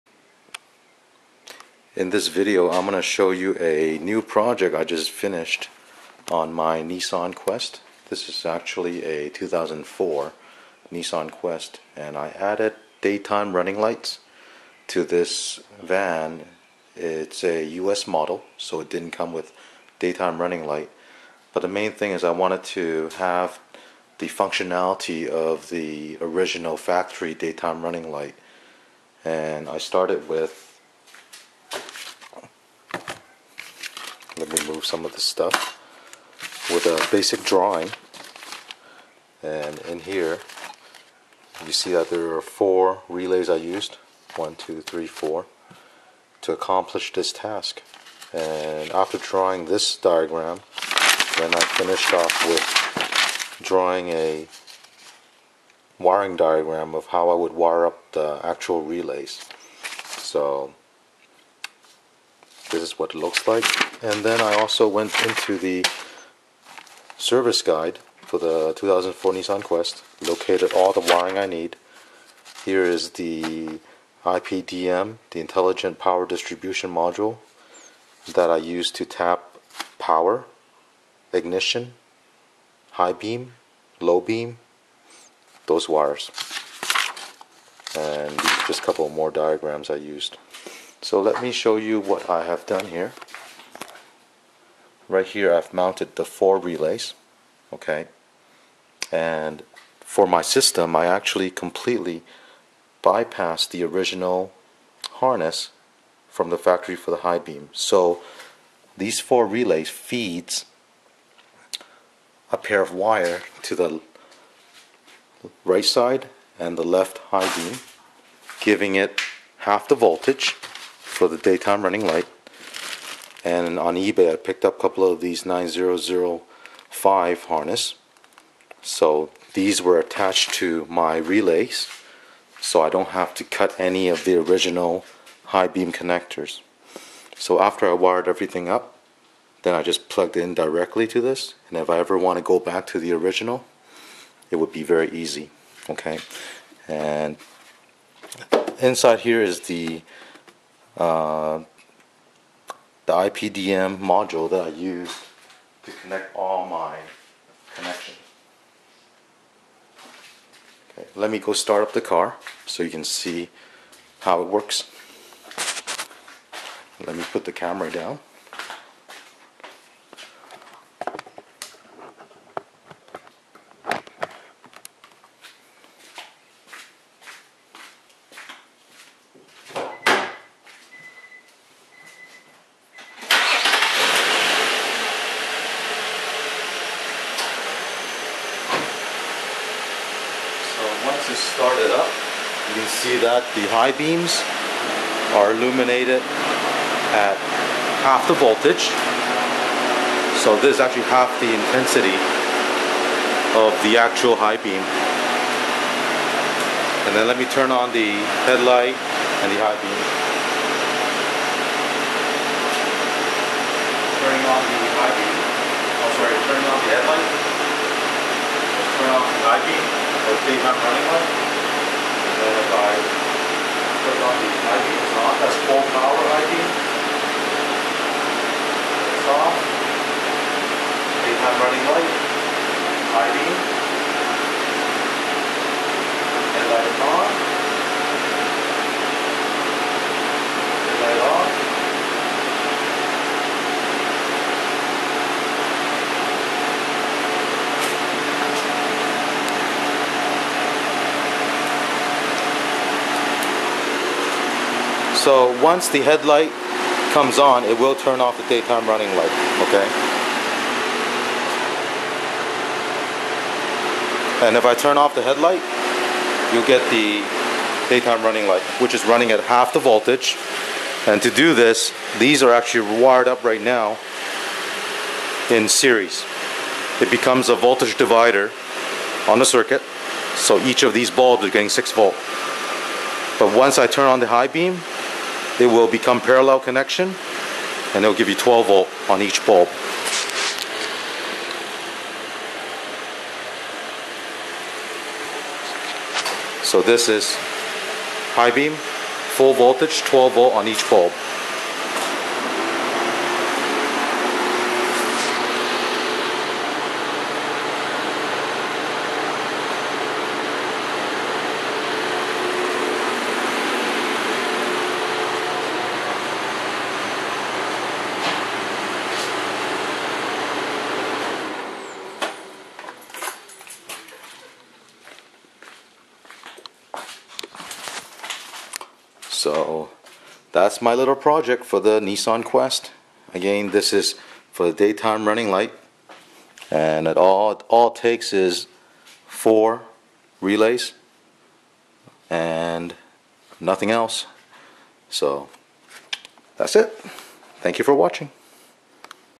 Custom Daytime Running Light Install - Works Like Factory DRL
I just finished this install which I designed and made myself to give my van DRL. It operates exactly like the factory ones where you have the high beams come on a half voltage for DRL, and full voltage when high beams are enabled. All this was done using 4 relays and couple of diodes. I also did not cut the factory harness so I can revert back whenever I want.

Depending on the make/model/year, the wiring can differ. This mod requires a person with some electrical knowledge to test all the wires before connecting. Now if you have a 2004 Nissan Quest, I recommend you find the service manual to for IPDM pinout.

2004 Quest IPDM connections:
Pin 7 (black/white) - +12V ign
Pin 28 (green) +12V High beam
Pin 30 (blue/silver dot) +12 V Low Beam
Pin 2 (black/yellow) +12 V constant power
Ground - find a good ground on the chassis